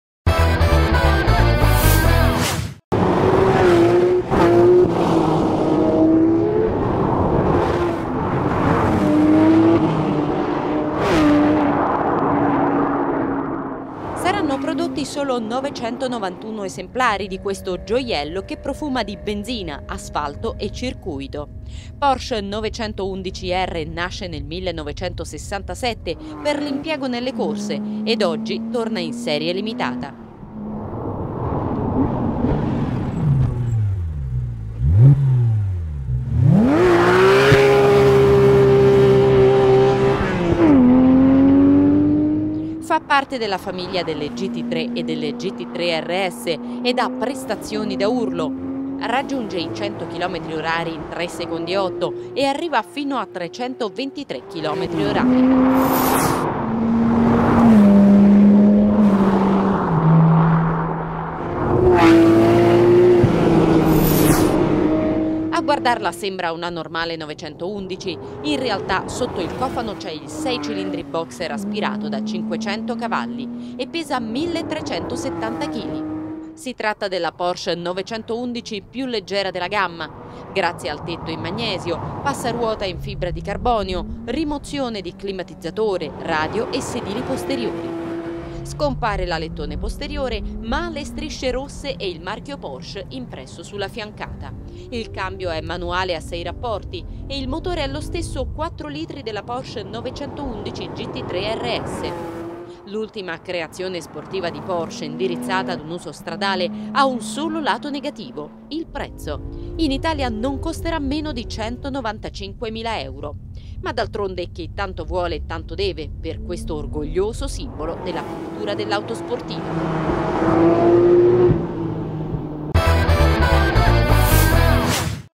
NEW PORSCHE 911 R 2016 - FIRST TEST DRIVE - ITA ENG SUB
NEW PORSCHE 911 R 2016 - PREVIEW FIRST TEST DRIVE - ITA ENG SUBTITLES

Subtitles available for all languages:
- Press ۞
- Press subtitles
- Choose Italian
- Press translate
- choose Language